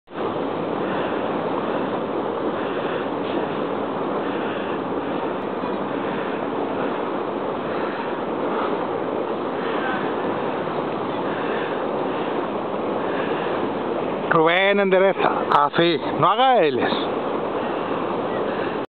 Navegando por el Gran lago de Portugal Alentejo
Un precioso barco de Alquiler, un Gps, dias de Vacaciones  y un precioso lago en el alentejo Portugues, unas vacaciones diferentes y divertidas... genial